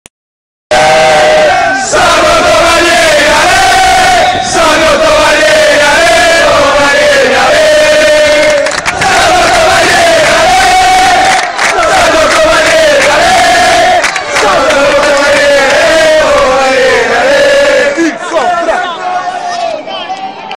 Bari Siena 2 1 Cori della curva Nord per Sandro Tovalieri 08 02 2014
TIFOSO DEL BARI? DIVENTA FAN

Bari: Guarna, Sabelli, Ceppitelli, Polenta, Calderoni, Defendi (42′ st Zanon), Romizi, Sciaudone, Galano (21′ st Lugo Martinez), Joao Silva, Nadarevic (31′ st Beltrame). A disposizione: Pena, Chiosa, Cani, Samnick, Varela, Del Vecchio. All. Alberti.

Siena: Farelli, Dellafiore, Ceccarelli (5′ st Cappelluzzo), Belmonte, Angelo (39′ st Thomas), Valiani, Pulzetti, Feddal, Rosina, Giacomazzi, Fabbrini (24′ st Spinazzola). A disposizione: Lamanna, Morero, Milos, Pamic, Schiavone, Vergassola. All. Beretta.

Arbitro: Manganiello di Pinerolo (Bagnoli-Carbone, Verdenelli).
Rete: 2′ pt Joao Silva, 46′ pt Defendi, 11′ st Cappelluzzo.
Recupero: 1′ pt, 3′ st.
Espulso: Valiani per gioco falloso 48′ st.
Ammoniti: Belmonte e Polenta per reciproche scorrettezze, Angelo, Cappelluzzo, Sciaudone e Beltrame per gioco falloso.